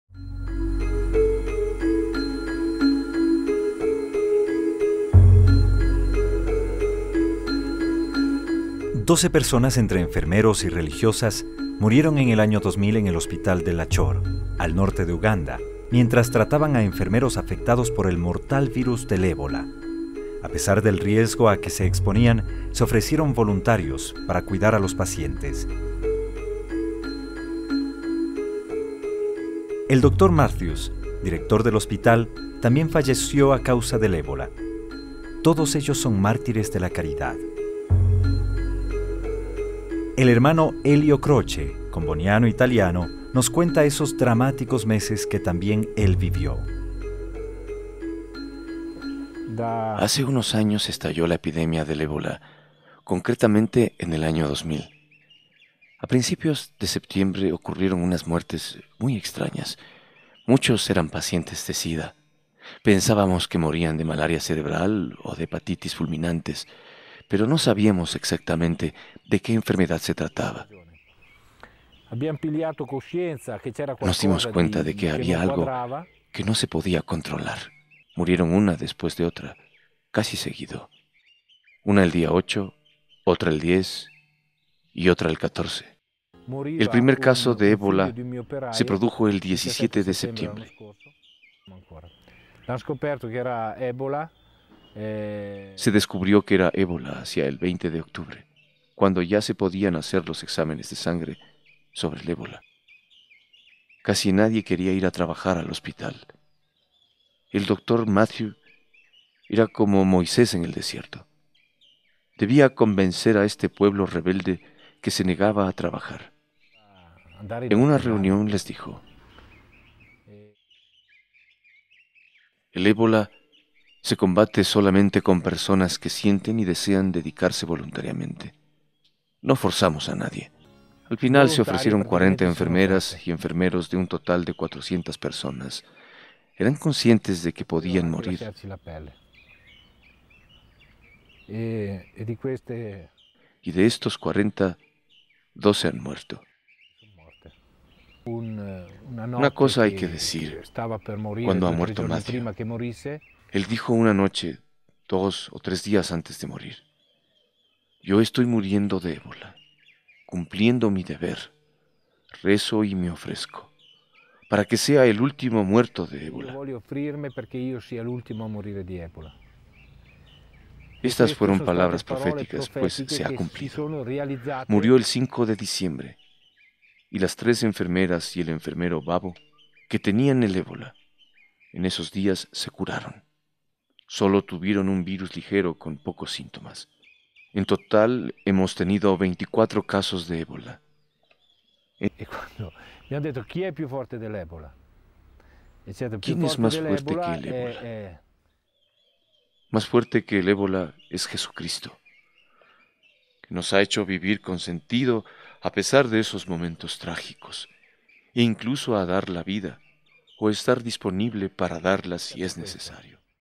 Mártires de la caridad
Un grupo de médicos y enfermeras atienden a personas afectadas por el ébola, pero esta mortal enfermedad les ataca y ellas mueren. testimonio del padre Elio que nos cuenta como tuvieron que enfrentar esta situación.